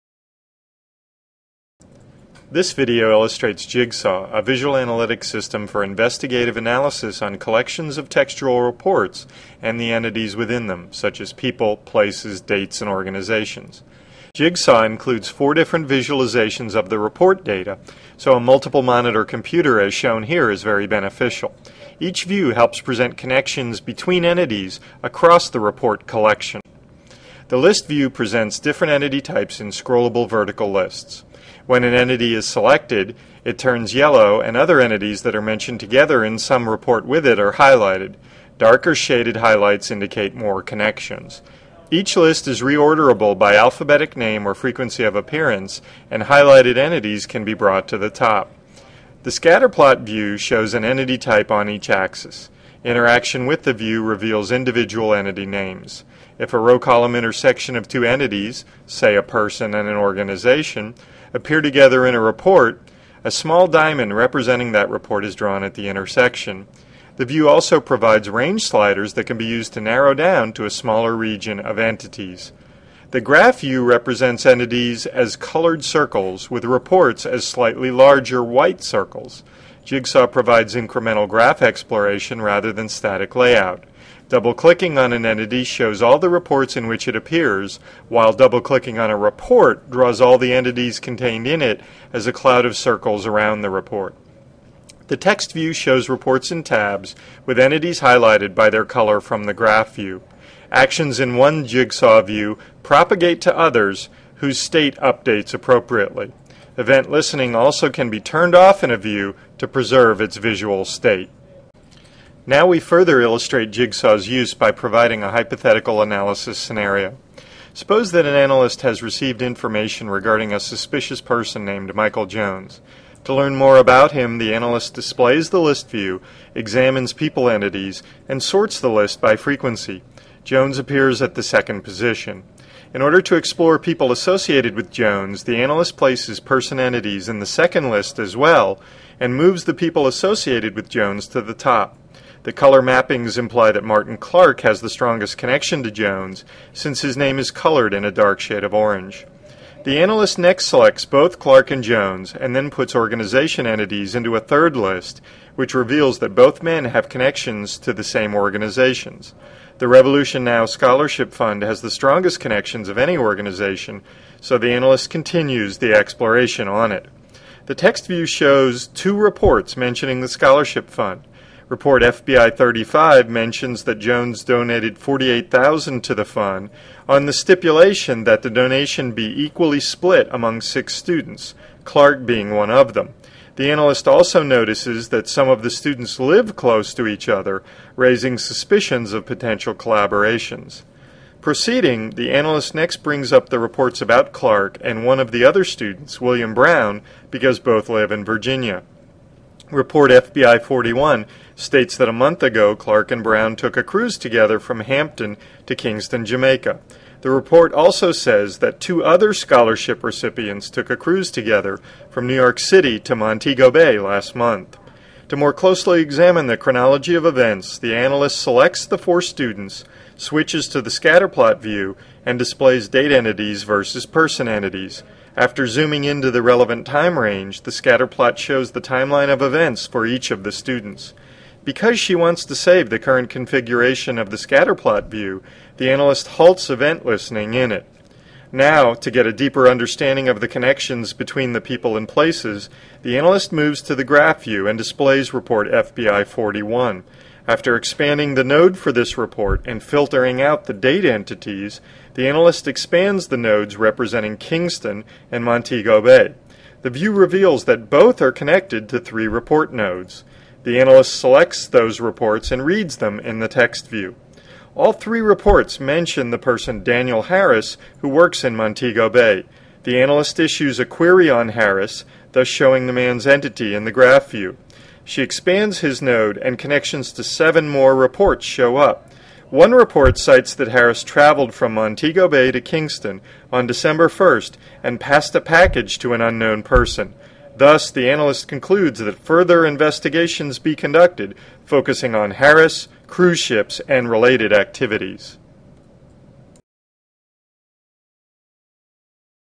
Jigsaw -- Visual Analytics
Using visualization and visual analytics to help investigative analysis and sensemaking.